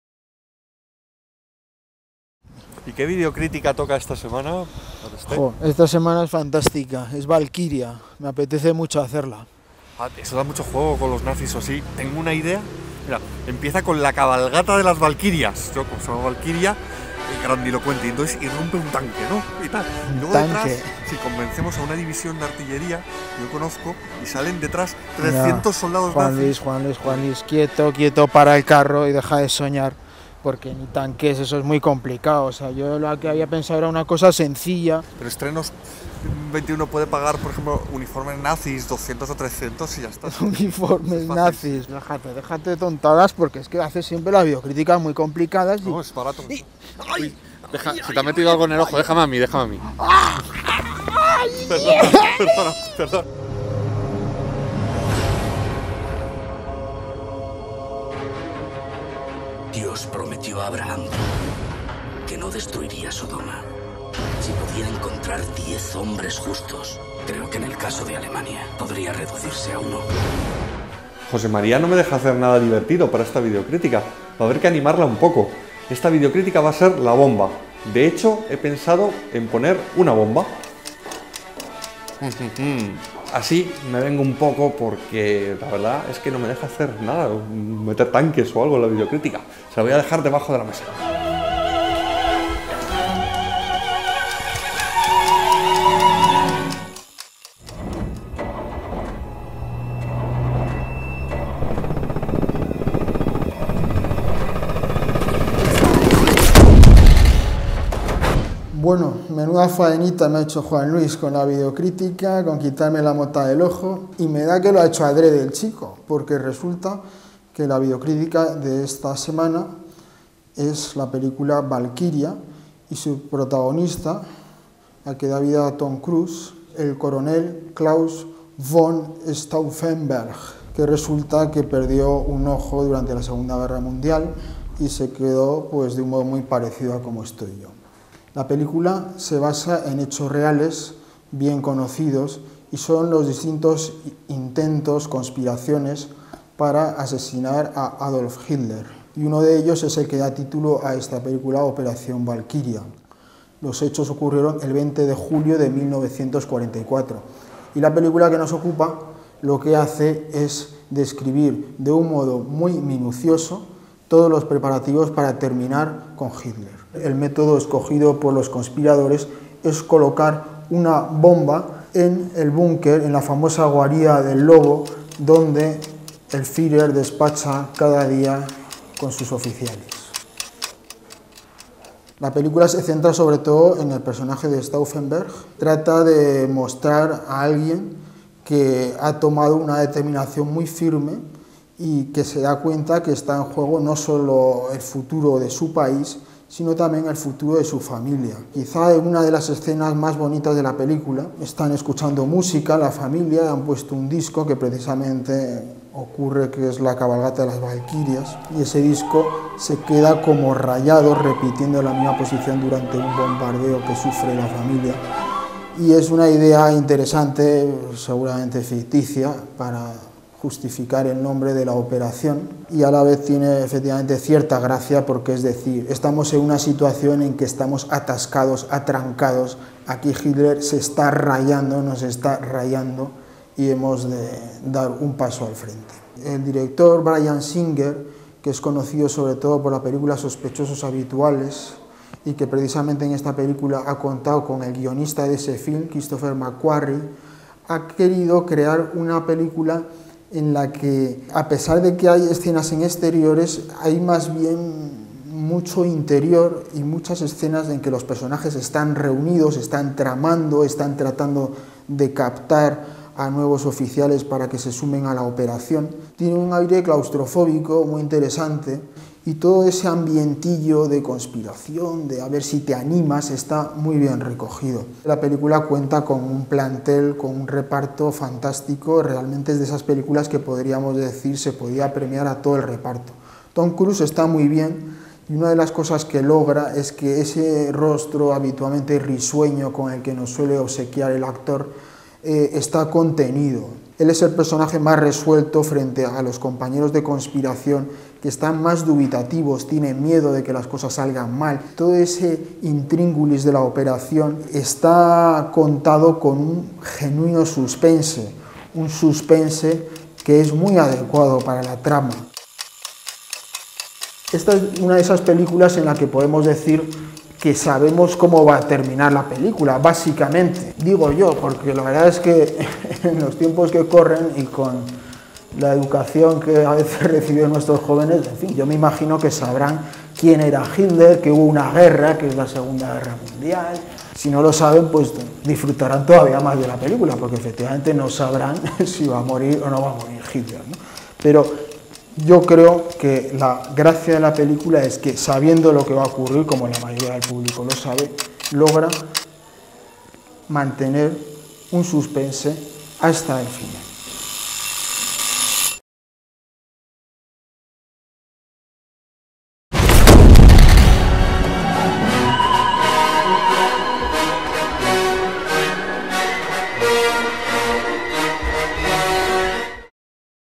Valkiria - Videocrítica de José María Aresté
No se pierdan el análisis de Valkiria del agudo José María Aresté, un crítico que demuestra tener mucho ojo para realizar su oficio. ¡Desde luego, la videocrítica de esta semana es un auténtica bomba de relojería! ¿No se lo creen?

¡Pues pinchen aquí y salgan de dudas!